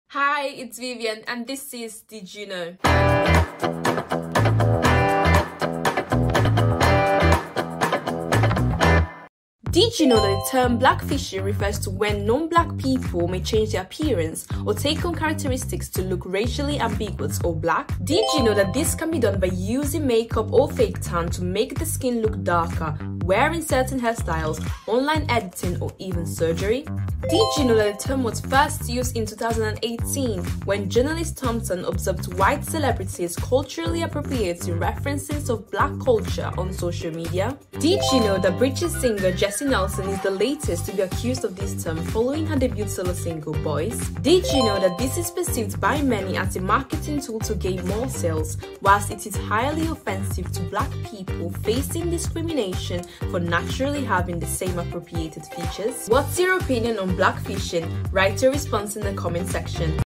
Blackfishing en videos musicales
¿Sabías que el término “Blackfishing” se refiere a cuando las personas que no son negras pueden cambiar su apariencia o adquirir una característica para verse racialmente ambiguas o negras? Muchos interpretan esto como una estrategia de marketing para ganar tracción entre nichos de mercado específicos. Sin embargo, la práctica es considerada ofensiva por las personas negras que, en realidad, enfrentan discriminaciones basadas en el color de su piel.

Nuevo vídeo de Sphera